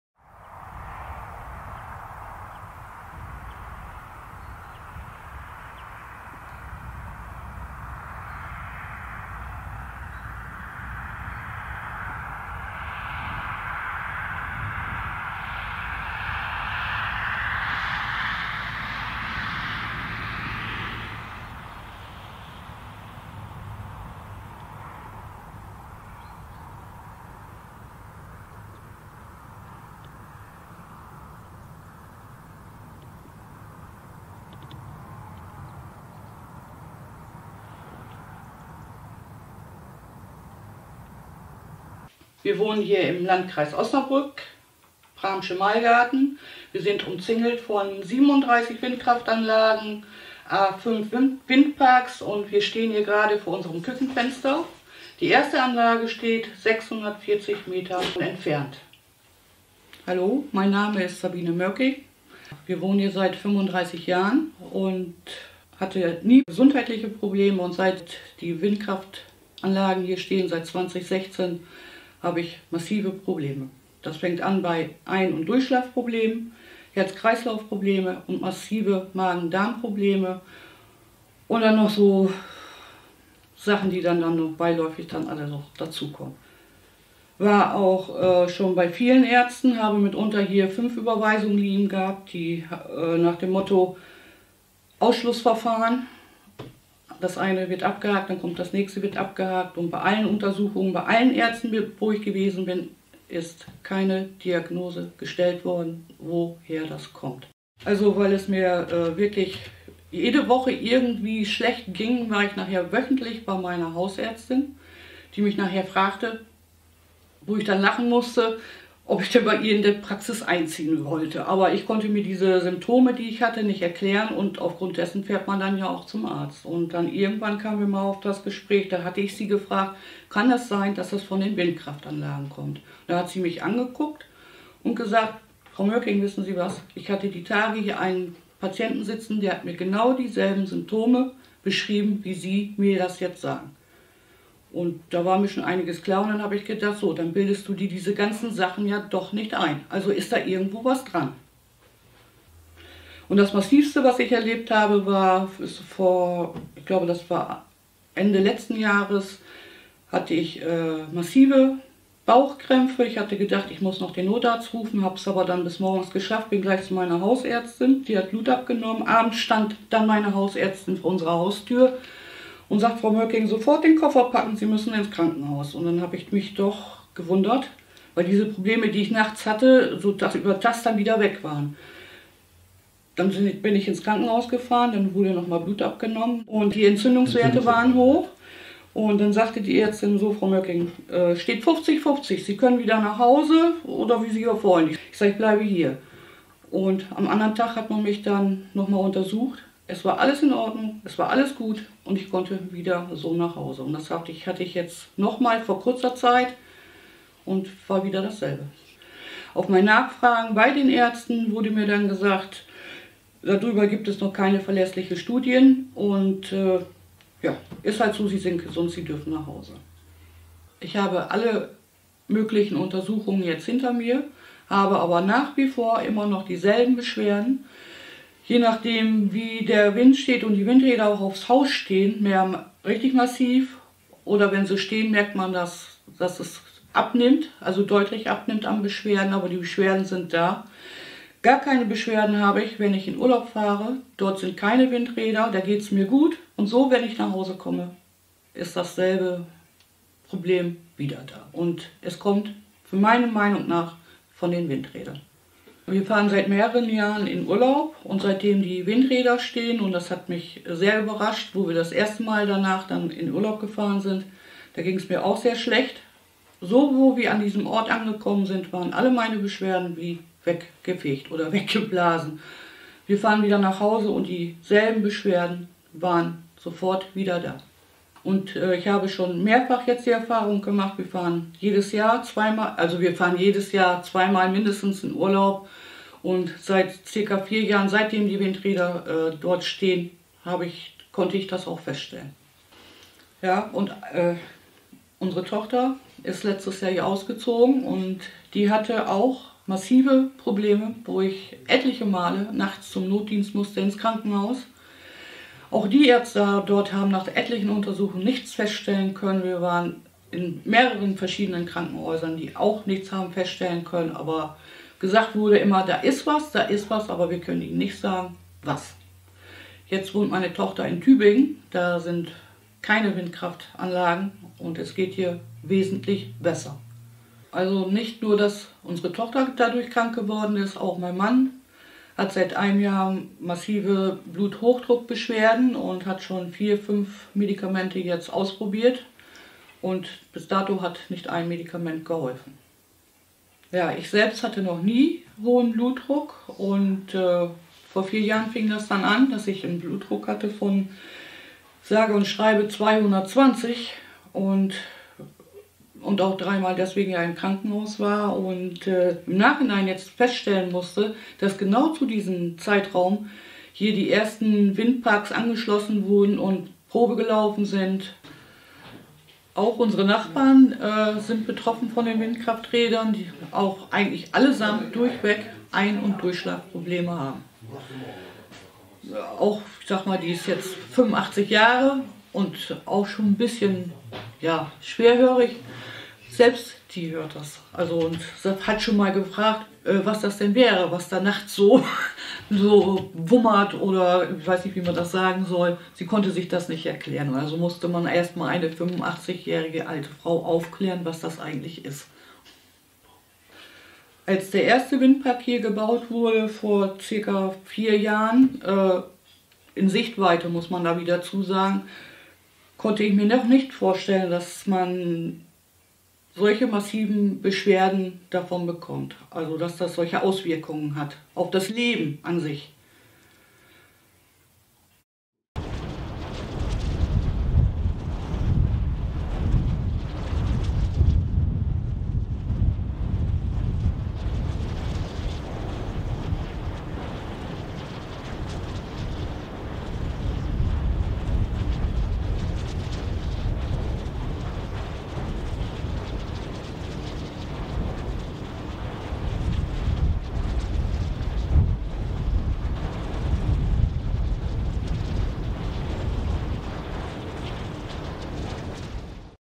Sick since a wind park was installed
Video by the German Association for Noise Pollution Protection of Humans and Animals (Deutsche Schutz-Gemeinschaft Schall für Mensch und Tier - DSGS e.V.).
English translation by NA-PAW.org — Subtitles by Friends-Against-Wind.org
“Unexplained medical emergencies and illness. As in the case of increased medical emergencies, accidents, and psychological symptoms with foehn (hot southerly winds on the northern slopes of the Alps [translator: similar to Chinooks]), the same is true for residents near wind turbines.”